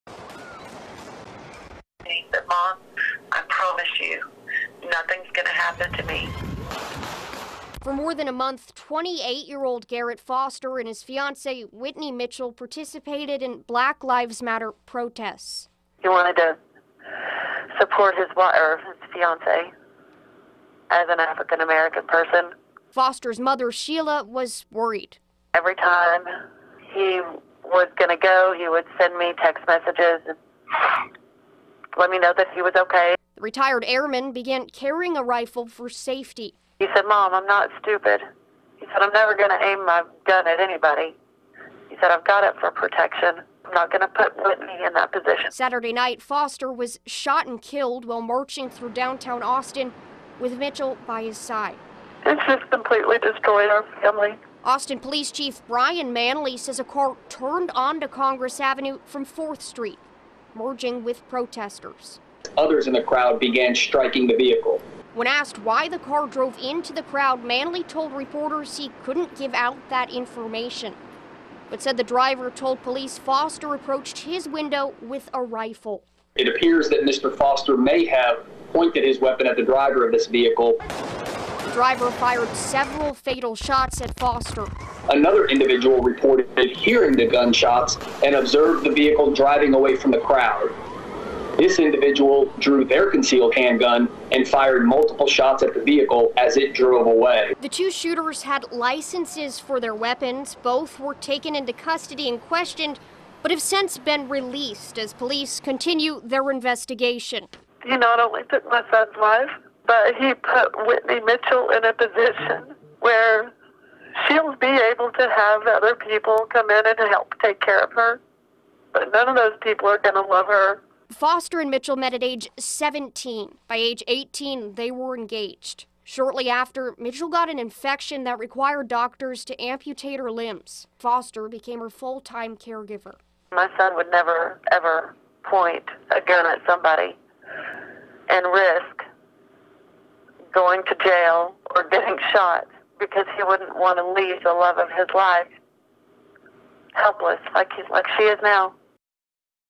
Mother of 28-year-old killed in Austin protest says he carried a rifle for safety
Garrett Foster was fatally shot by a man driving through the protest. STORY